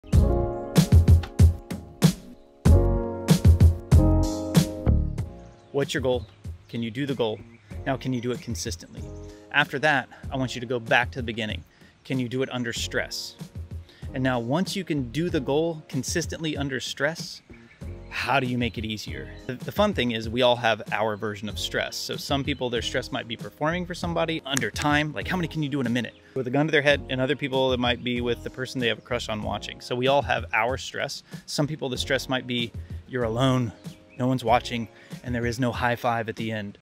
Go to stress don’t let it come to you. I learned that stress is a good tool for skill and wisdom
Whats the stress that breaks your consciousness, grace, or skill? It may be your best friend if you learn to change your story and slowly train under that stress.